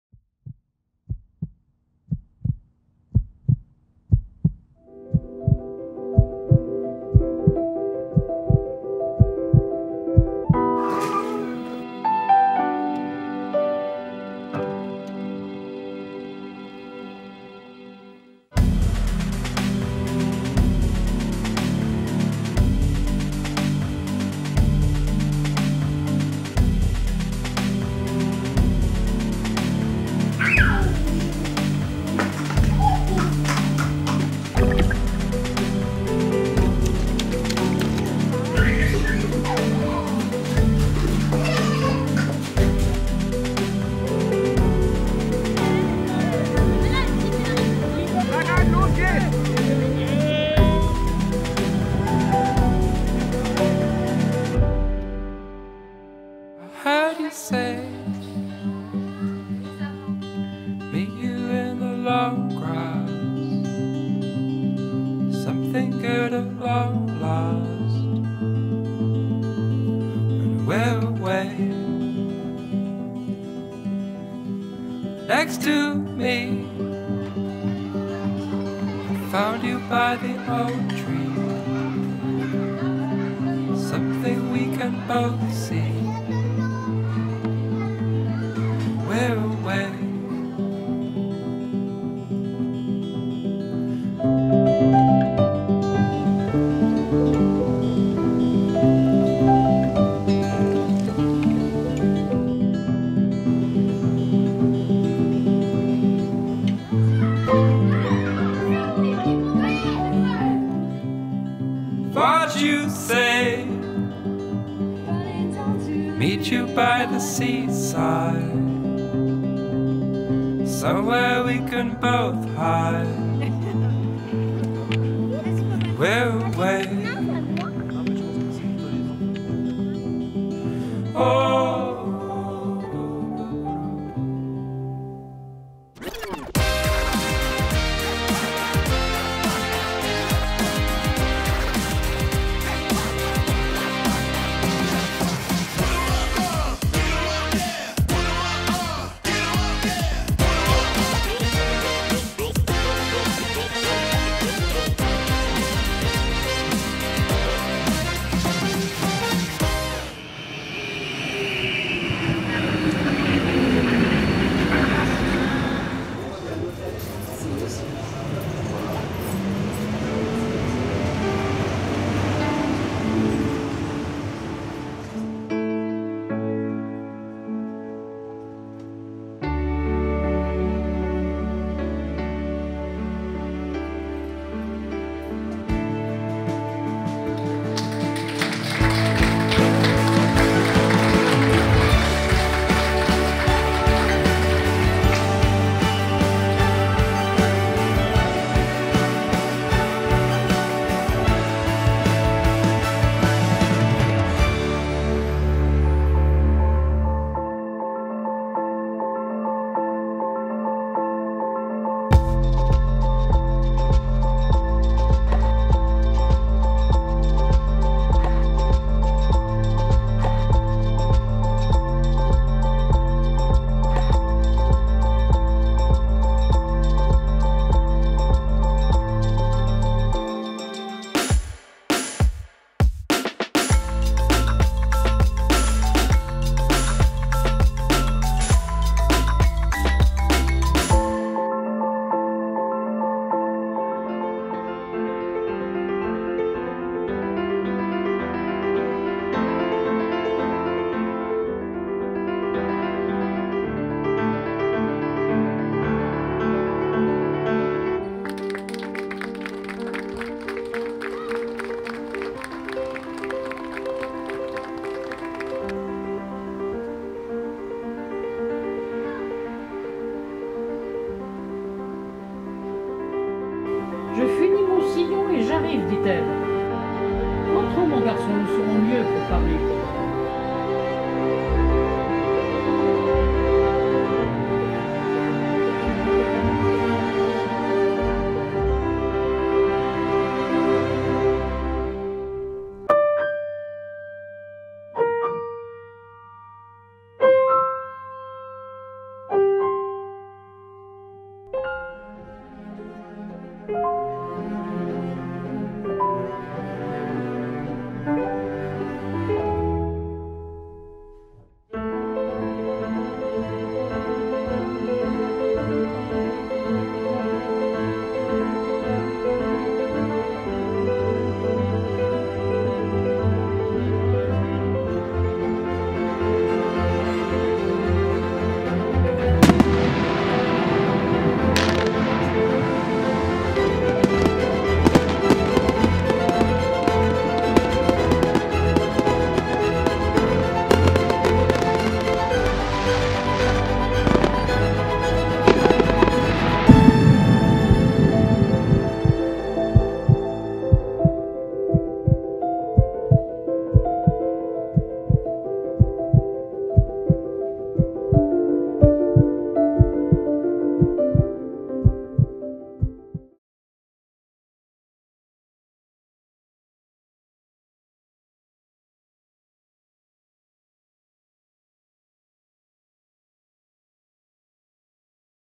Vélizy-Villacoublay, ville intergénérationnelle
Nouveau film de présentation de la ville de Vélizy-Villacoublay autour de l'intergénérationnel.
Merci à tous les véliziens qui y ont participé !